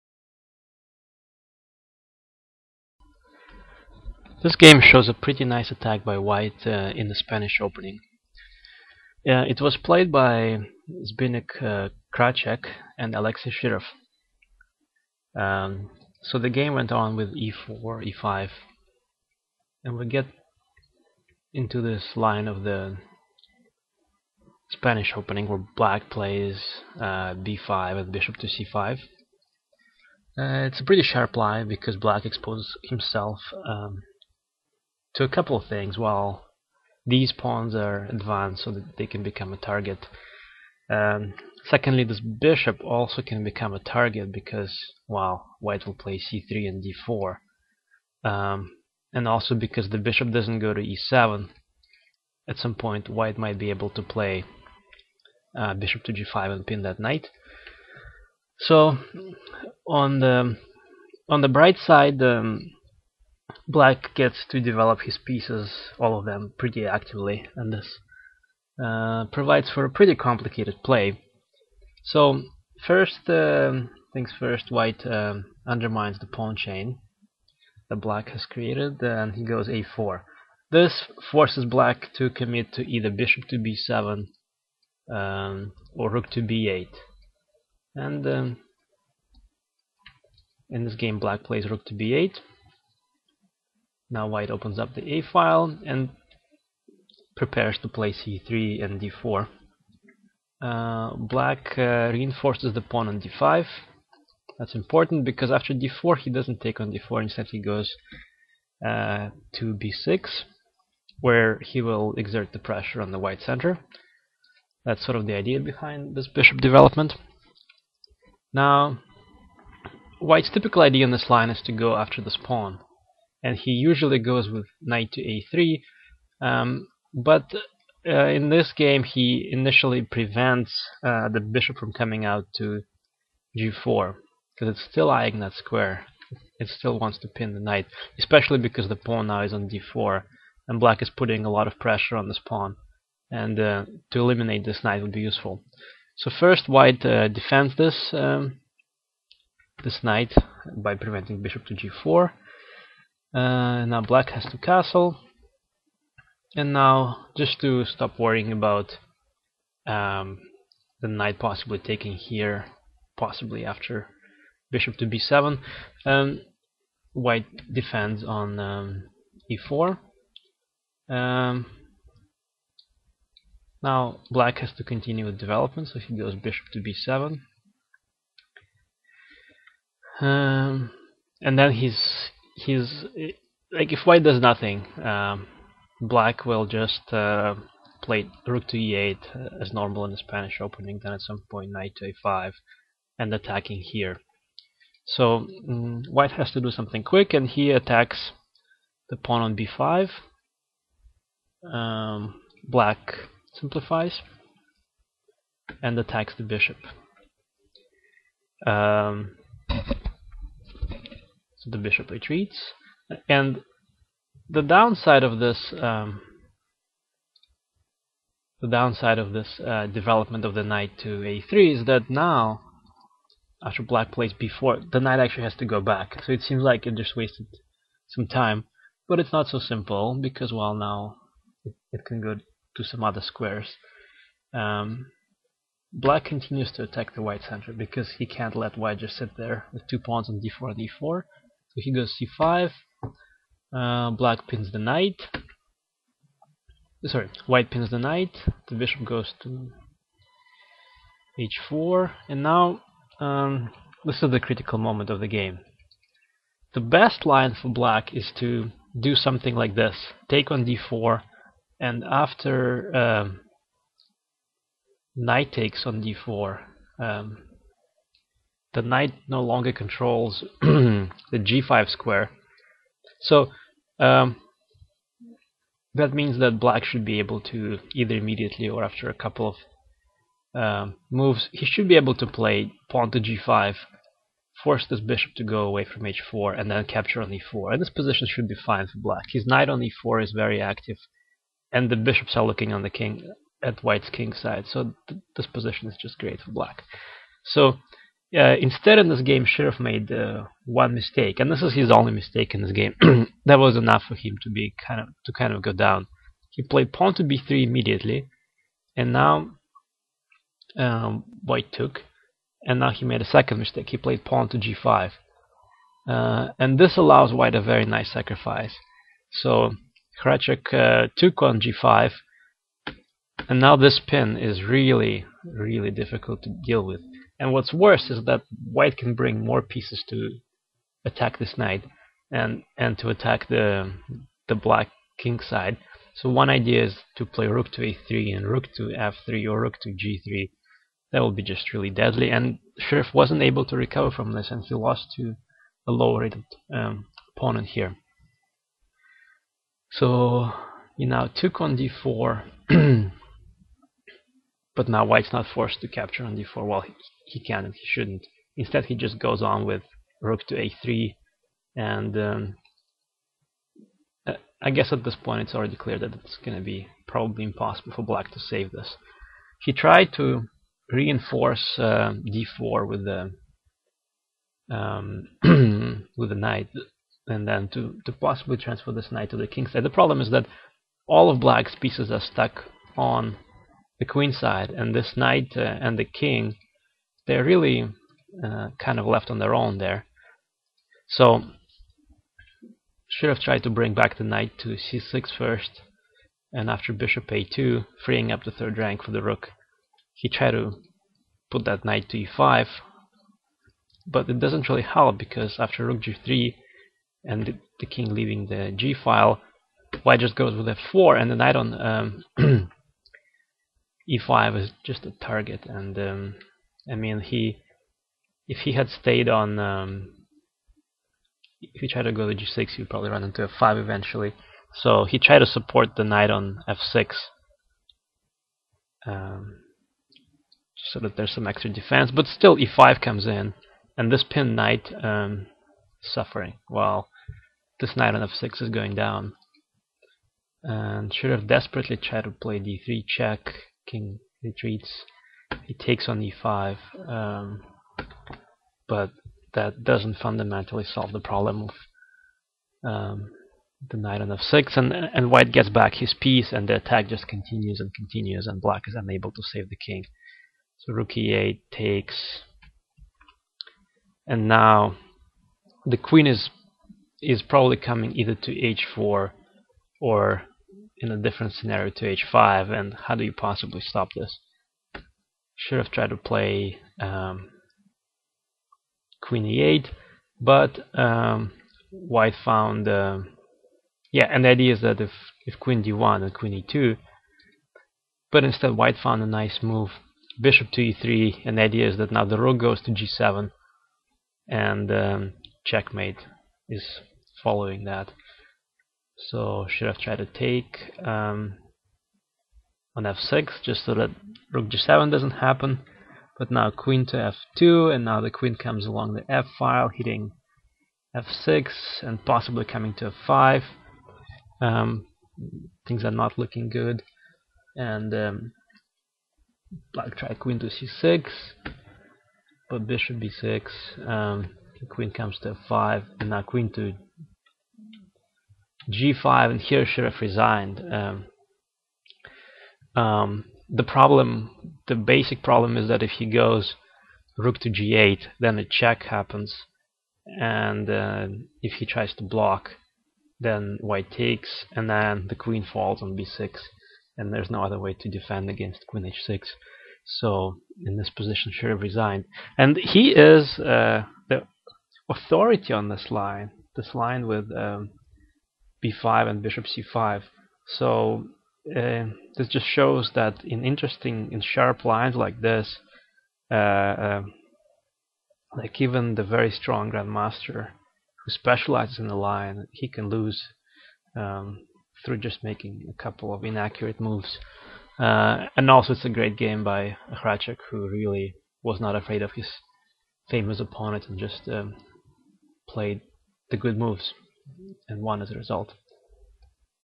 Piece Sacrifice For Attack, Shirov Defeated by his own weapon
Shirov - Hracek, 1997. Alexei Shirov is a brilliant attacker, and is a leading opening expert in this line of the Spanish opening, but here he gets blown off the board with a piece sacrifice.